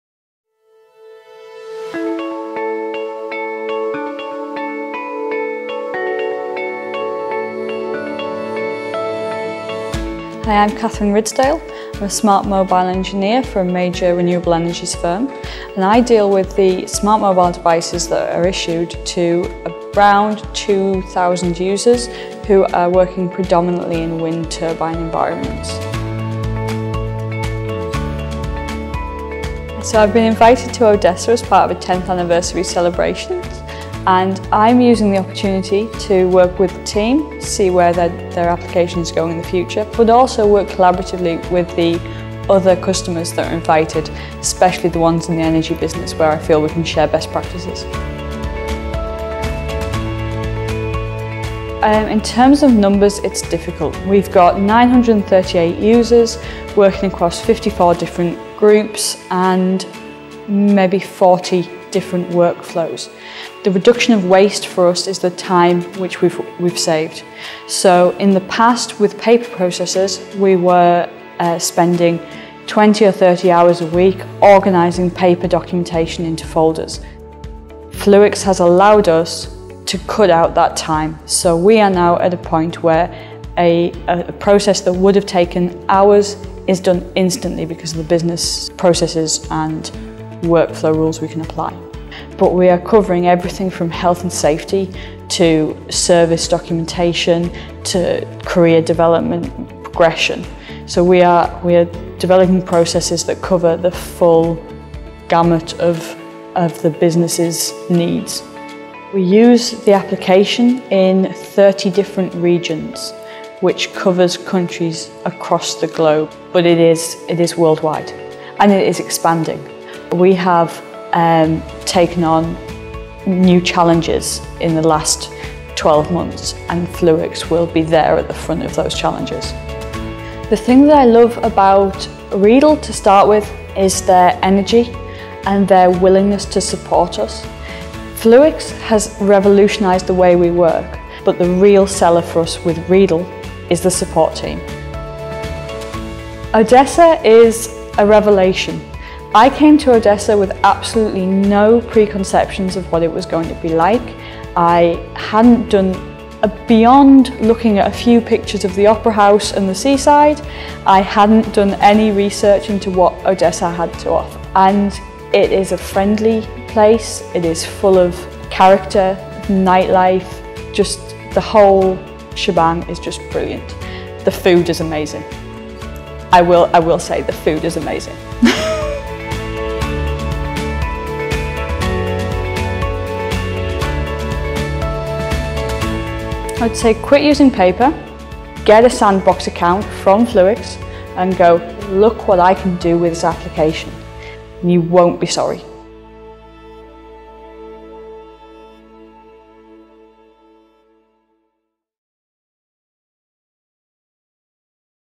How Siemens Gamesa automated business processes with Fluix
Siemens Gamesa shared experience how they reduced cycle times and paper costs with Fluix mobile software solution.